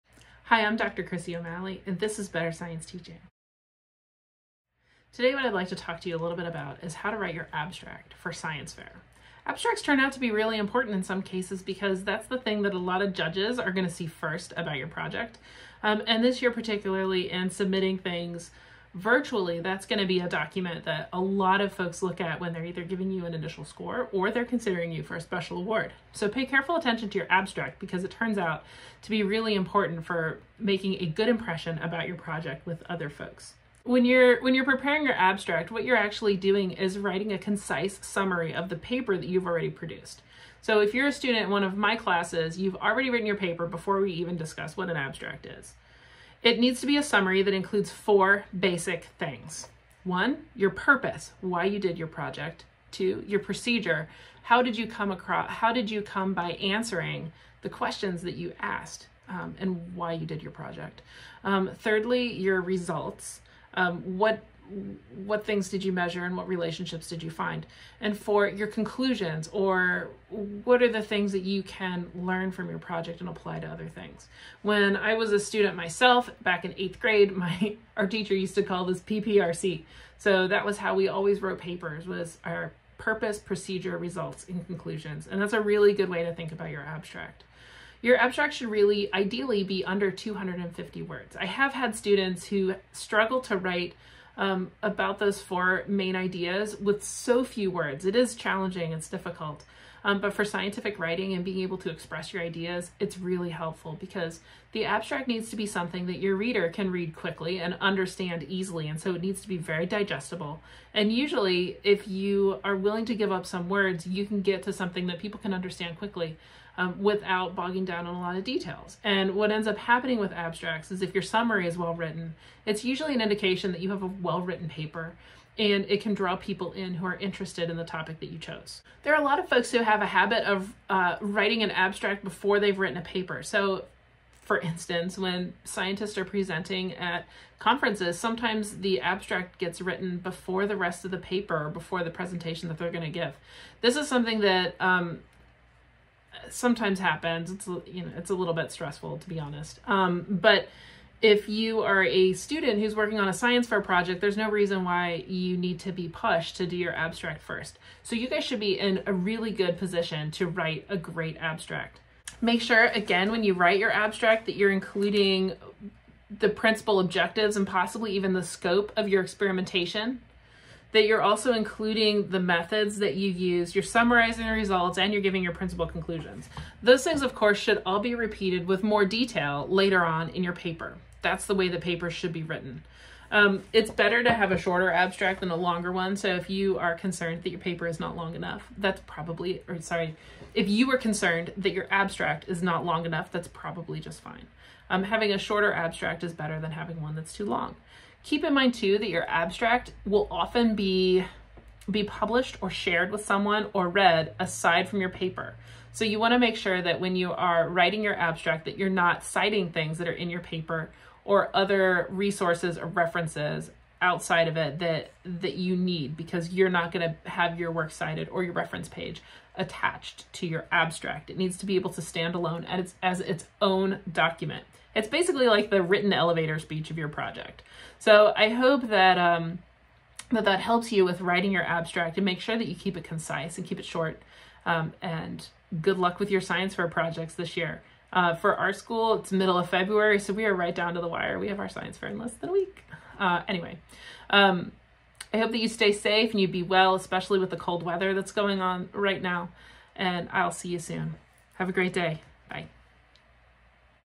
Write your Science Fair Abstract | Science Fair Friday
Better understand the critical components of your Science Fair Abstract and learn PPRC- purpose, procedure, results, conclusion.